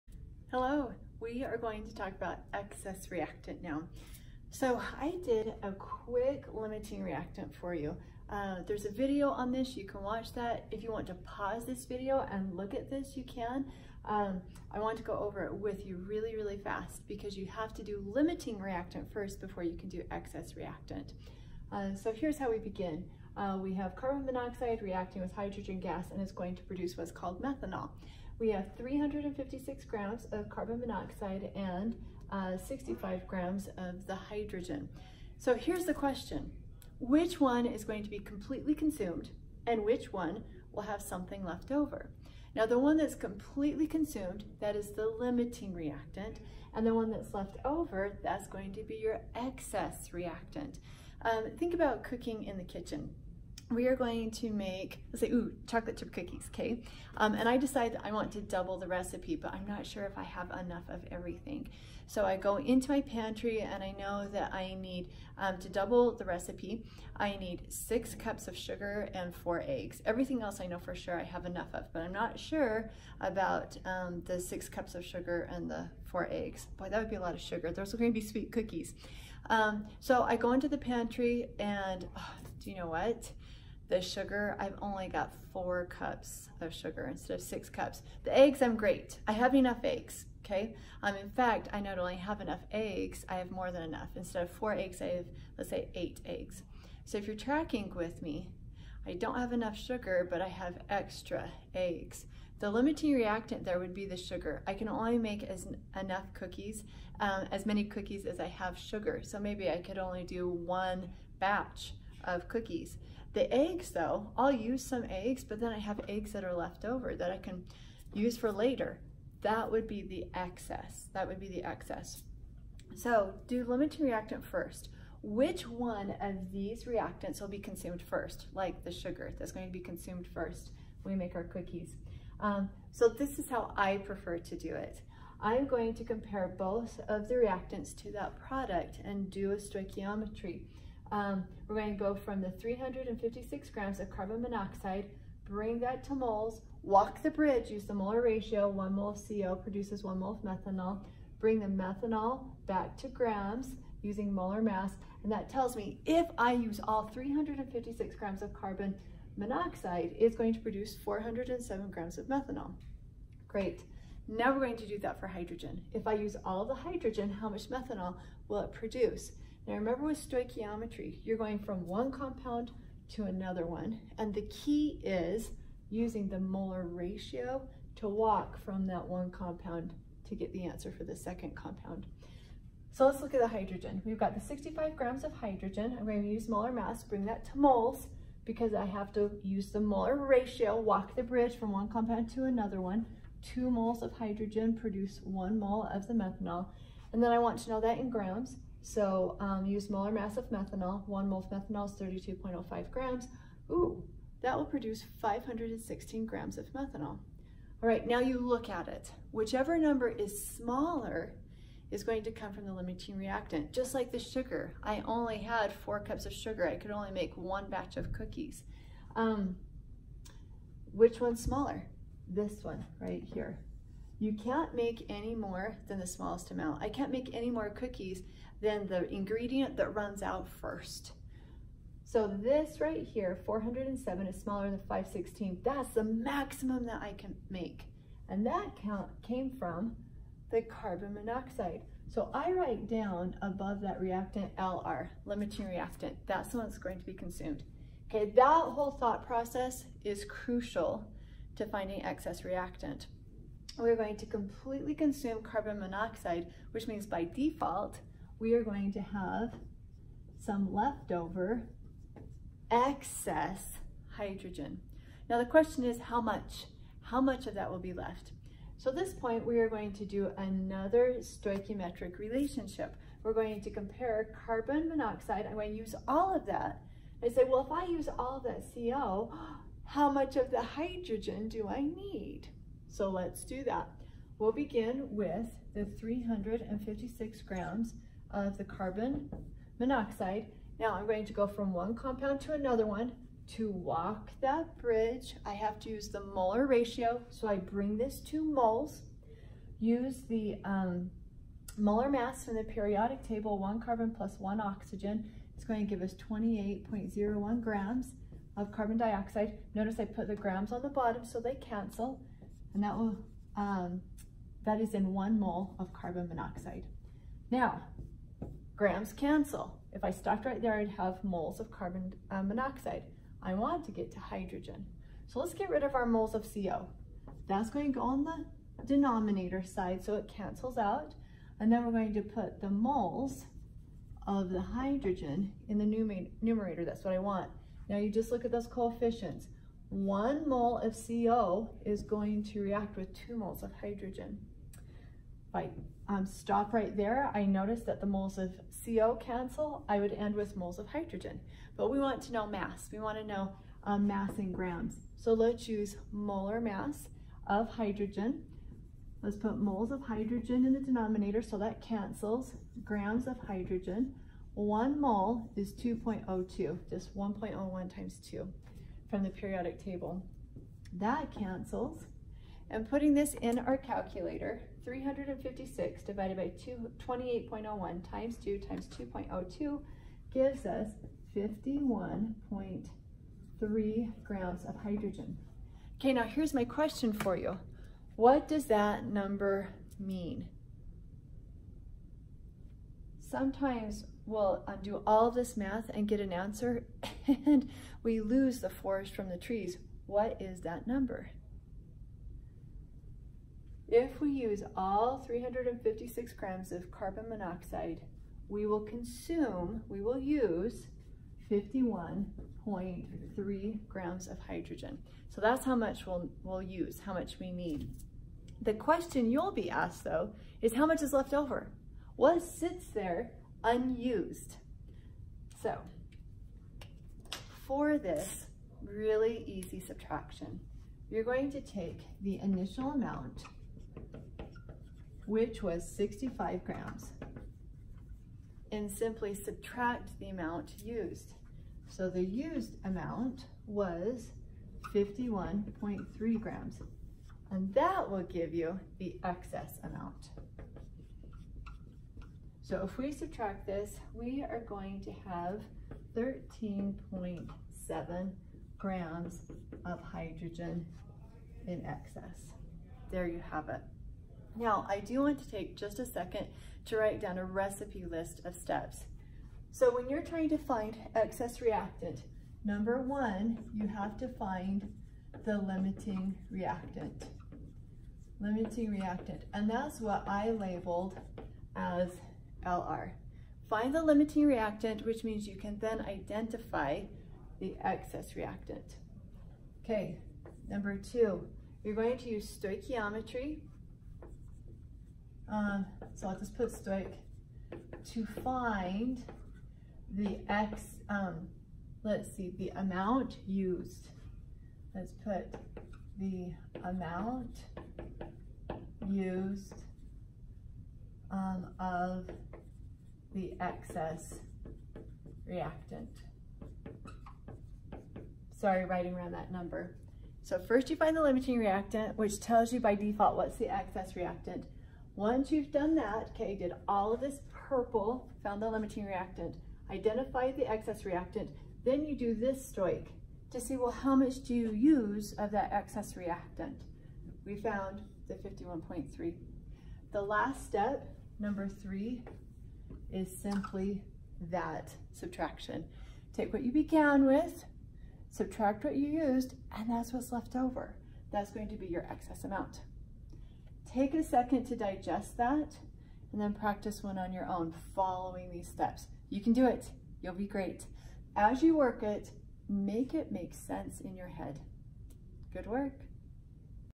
Excess Reactant
How to determine and calculate the excess reactant.  Calculate both the amount used and the amount remaining for an excess reactant.  Example provided.